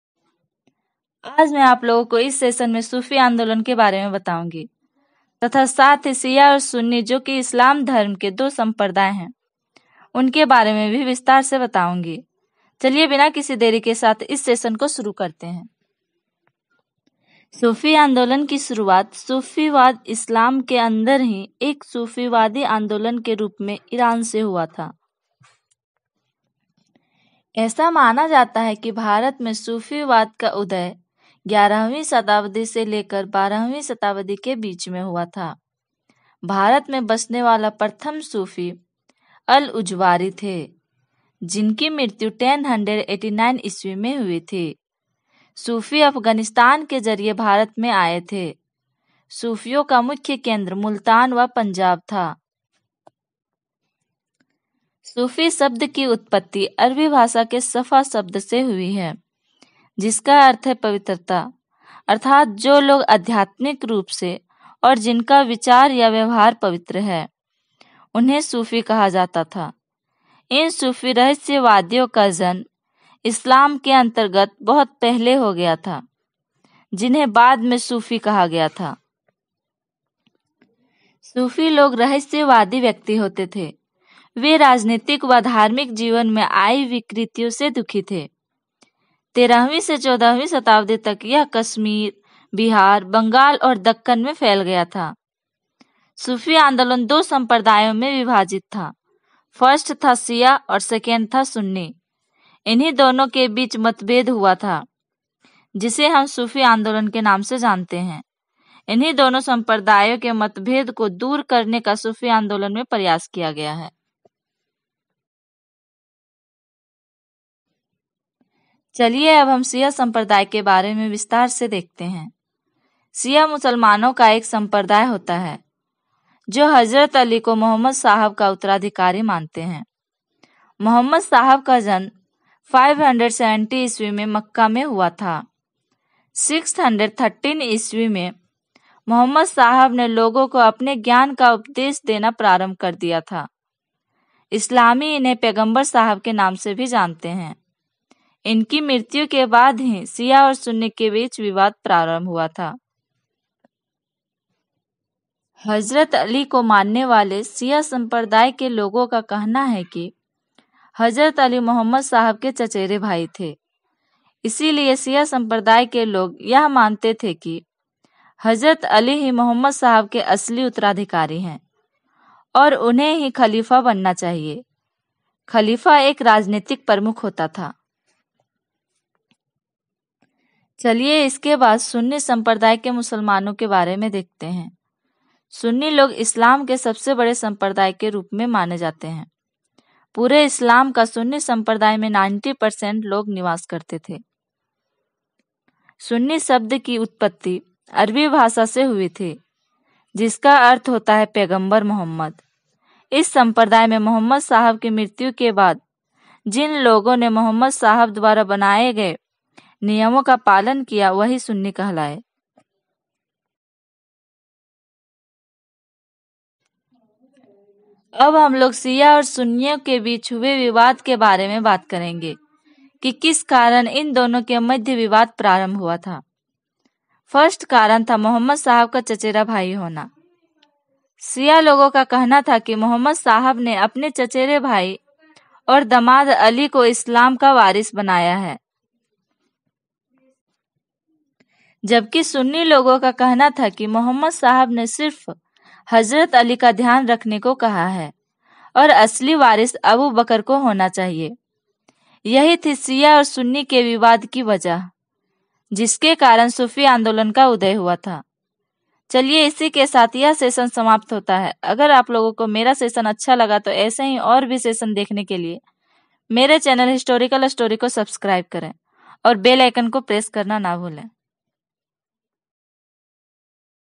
सूफ़ी आन्दोलन || Sufi movement || History of sufism || सूफीवाद का इतिहास हिंदी में
सूफ़ी आन्दोलन : - सूफ़ी आन्दोलन की शुरुआत सूफीवाद इस्लाम के भीतर ही एक सुधारवादी आंदोलन के रूप में ईरान से हुआ था |

11वीं से 12वीं शताब्दी के बिच इसका उदय हुआ था |

13वीं से 14वीं शताब्दी
                                यह कश्मीर,बिहार बंगाल और दक्कन तक फैला हुआ था |